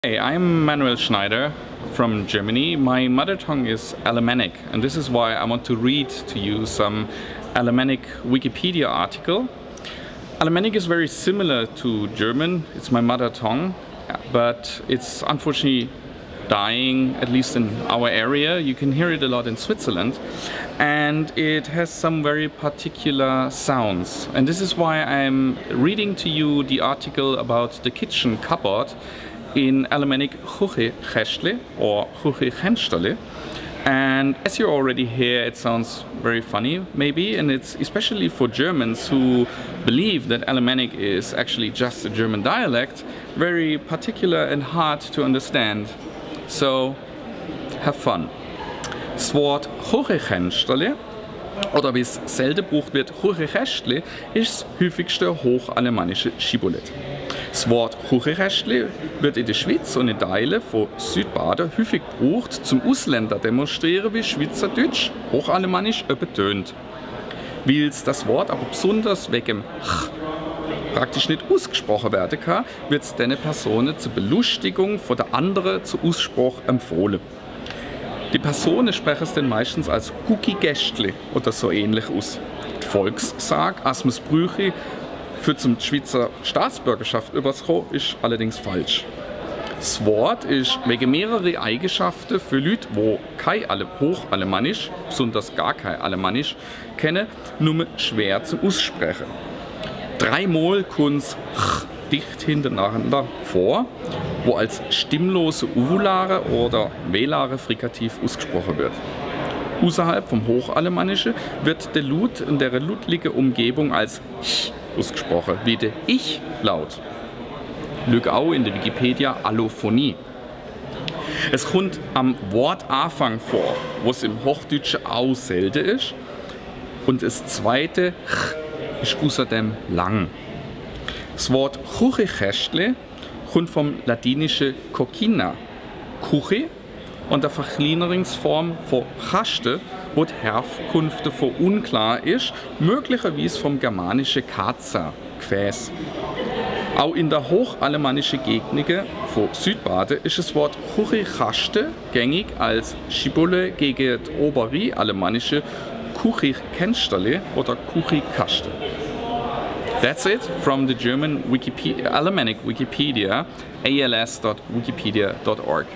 Chuchichäschtli (Alemannic) - Manuel Schneider - WikiReadings
Chuchichäschtli from the Alemannic Wikipedia read by Manuel Schneider.
Correct on: 10/08/14